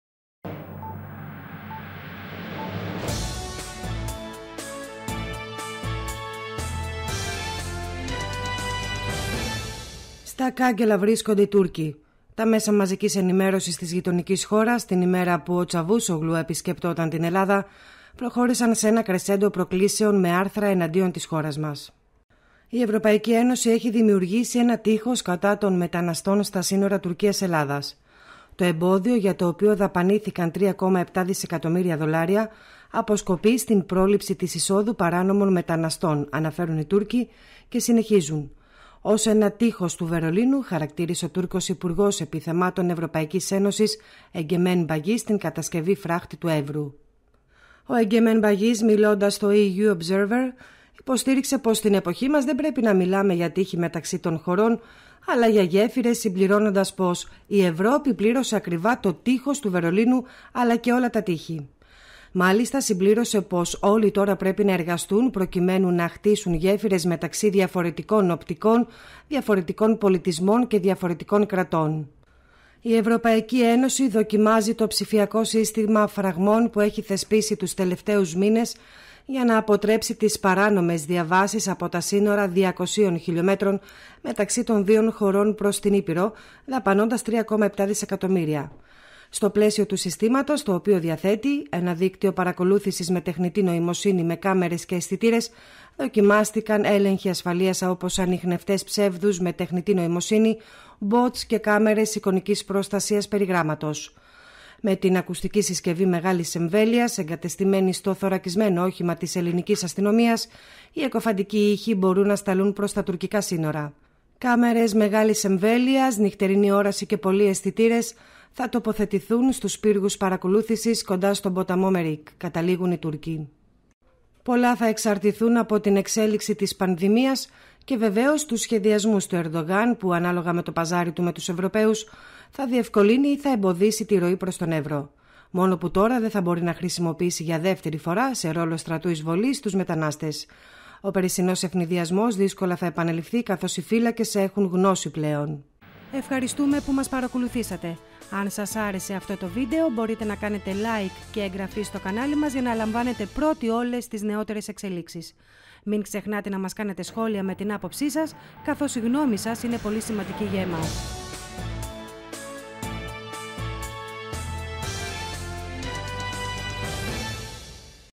"Οι Έλληνες τοποθέτησαν υπερόπλα στον Έβρο"
''Κάμερες μεγάλης εμβέλειας, νυχτερινή όραση και πολλοί αισθητήρες έχουν τοποθετηθεί στους πύργους παρακολούθησης''
 Στα ''κάγκελα'' βρίσκονται οι Τούρκοι. Τα ΜΜΕ της γειτονικής χώρας, την ημέρα που ο Τσαβούσογλου επισκεπτόταν την Ελλάδα, προχώρησαν σε ένα κρεσέντο προκλήσεων, με άρθρα εναντίον της χώρας μας!

Τι αναφέρουν

''Η ΕΕ έχει δημιουργήσει, ένα τείχος κατά των μεταναστών στα σύνορα Τουρκίας-Ελλάδας. Το εμπόδιο, για το οποίο δαπανήθηκαν 3,7 δισεκατομμύρια δολάρια, αποσκοπεί στην πρόληψη της εισόδου παράνομων μεταναστών'', αναφέρουν οι Τούρκοι.

Η περυσινή εμπειρία, της καθοδηγούμενης από τις τουρκικές αρχές, απόπειρας μαζικής εισβολής παράτυπων μεταναστών και προσφύγων στο τελωνείο των Καστανιών, έχει κάνει πιο «σοφές» τις επιφορτισμένες με τη θωράκιση του Έβρου αρχές και η αμυντική θωράκιση ανέβηκε επίπεδα.

Βέβαια, όπως ήταν αναμενόμενο αυτός ο στραγγαλισμός  των συνόρων μας, έναντι των λαθρομεταναστών που προωθούνται από την Τουρκία δεν άργησε να φέρει αντιδράσεις.

Και συνεχίζουν:

''Ως νέο «Τείχος του Βερολίνου», χαρακτήρισε ο Τούρκος υπουργός επί θεμάτων Ευρωπαϊκής Ένωσης, Εγκεμέν Μπαγίς, την κατασκευή φράχτη του Έβρου.

Ο Εγκεμέν Μπαγίς, μιλώντας στον «EU Οbserver» υποστήριξε πως στην εποχή μας δεν πρέπει να μιλάμε για τείχη μεταξύ των χωρών, αλλά για γέφυρες, συμπληρώνοντας πως, η Ευρώπη πλήρωσε ακριβά το Τείχος του Βερολίνου, αλλά και όλα τα Τείχη.

Μάλιστα συμπλήρωσε, πως όλοι τώρα πρέπει να εργαστούν προκειμένου, να «χτίσουν γέφυρες μεταξύ διαφορετικών οπτικών, διαφορετικών πολιτισμών και διαφορετικών κρατών».''

Η απρόσμενη υβριδική επίθεση, διαφοροποίησε το υφιστάμενο δόγμα αποτροπής, με την πιο ενεργό εμπλοκή πλέον του ευρωπαϊκού παράγοντα (Frontex) και την ενίσχυση των τεχνικών σχεδίων και μέσων «επί του πεδίου».

Ωστόσο αρμοδίως, δεν καλλιεργούνται ψευδαισθήσεις ότι με την ύψωση και ενίσχυση του τείχους, το πέρασμα του Έβρου θα κλείσει «αεροστεγώς» για τις ροές.

Μπορεί να περιορίσει σημαντικά τις διελεύσεις, όχι όμως και να τις εξαλείψει, εξαιτίας της συνεχούς μεταβολής της κοίτης του ποταμού, πολύ περισσότερο που στην πλειονότητα  των περιπτώσεων, κατευθύνονται και υποβοηθούνται στο εγχείρημα της διέλευσης του ποταμού, από την τουρκική συνοριοφυλακή.

Η οποία, επινοεί τεχνάσματα για να διευκολύνει την αποβίβασή τους στην ελληνική όχθη, με ένα εξ αυτών, που καταγράφεται σχεδόν καθημερινά όταν πέφτει το σκοτάδι, να αποτελούν οι πυροβολισμοί στον αέρα ή και προς εκφοβισμό των μεικτών ελληνοευρωπαϊκών περιπόλων σε κάποιο σημείο του για αντιπερισπασμό, με την ταυτόχρονη διέλευση ομάδων μεταναστών, σε άλλη, αφύλακτη, διάβαση.

Τα αυξανόμενα μέτρα φρούρησης, δεν δείχνουν να πτοούν τους απελπισμένους μετανάστες, οι οποίοι μέσω σημείων του ποταμού που δύσκολα φρουρούνται, εξακολουθούν να περνούν στο ελληνικό έδαφος και διασχίζοντας τον ορεινό όγκο του Εβρου και της Ροδόπης, προωθούνται στα ενδότερα.

Ως κυρίως διάδρομος, για την παράκαμψη της φρουρούμενης από την ΕΛ.ΑΣ. Εγνατίας Οδού, χρησιμοποιείται η περιοχή κοντά στα χωριά Μικρό και Μεγάλο Δέρειο, οι κάτοικοι των οποίων διαμαρτύρονται ότι εισβάλλουν στα σπίτια τους και στις αποθήκες προκαλώντας κλοπές και ζημιές.

Σε αρκετές περιπτώσεις, έχει καταγγελθεί η κλοπή αγροτικών αυτοκινήτων, τα οποία οι μετανάστες εγκαταλείπουν σε κάποια σημεία της διαδρομής στις πλαγιές της Ροδόπης και της Ξάνθης.

Πολλοί εξακολουθούν να συλλαμβάνονται στη διαδρομή προς τη Θεσσαλονίκη, κάποιοι άλλοι όμως, αρκετοί, πιθανότατα, καταφθάνουν στα αστικά κέντρα με την ελπίδα ότι θα συνεχίσουν προς την Ευρώπη.

Με τη βελτίωση των καιρικών συνθηκών, αναμένεται να επαναλειτουργήσει και ο θαλάσσιος δρόμος του Βορείου Αιγαίου, διά του οποίου τα τελευταία χρόνια καταφθάνουν χιλιάδες αλλοδαποί.

Καθώς μπαίνουμε στα βαθιά της άνοιξης και πλησιάζει το καλοκαίρι και η κοίτη του Εβρου θα υποχωρήσει, τα προσφυγικά καραβάνια ετοιμάζονται για την προώθησή τους στη χερσαία πύλη προς την Ευρώπη.

Και καταλήγουν οι Τούρκοι

''Η ΕΕ δοκιμάζει, το ψηφιακό σύστημα φραγμών που έχει θεσπίσει τους τελευταίους μήνες, για να αποτρέψει τις παράνομες διαβάσεις από τα σύνορα 200 χιλιομέτρων μεταξύ των δύο χωρών προς την ήπειρο, δαπανώντας 3,7 δισεκατομμύρια δολάρια.